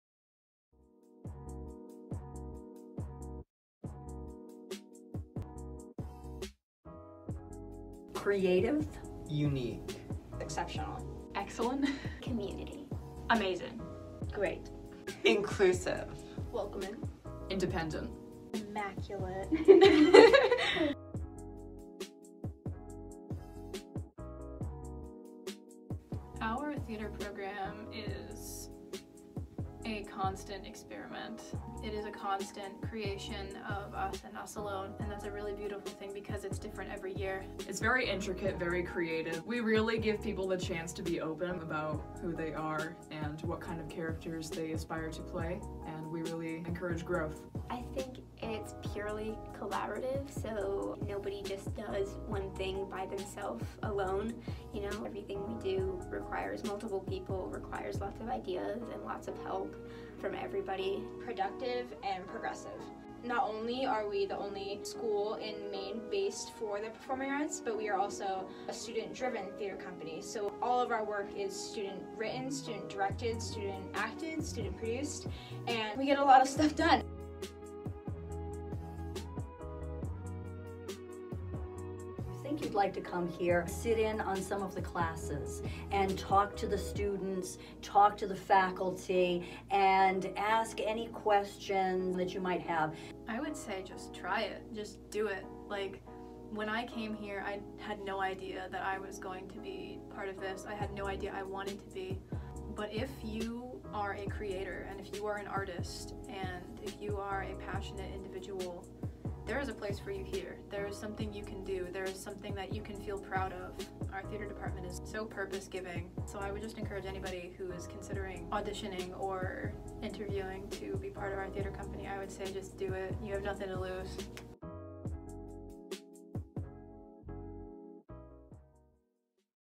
MeAA Theatre Program Student Interview
Lexi Alcott interviewed our theater students this year and created this 2 minute video about the theater program at MeAA